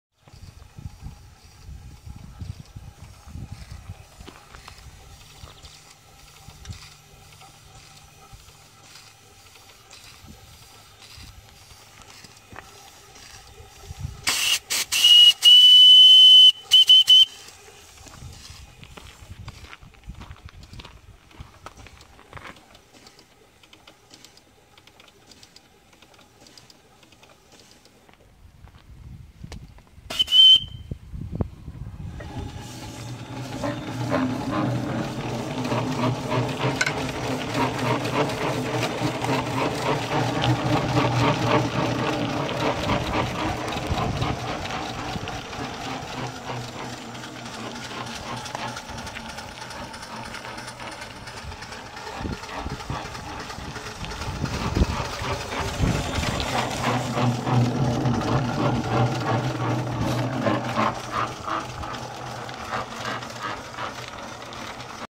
3inch traction engine
Newly finished 4 shaft twin cylinder 3speed traction engine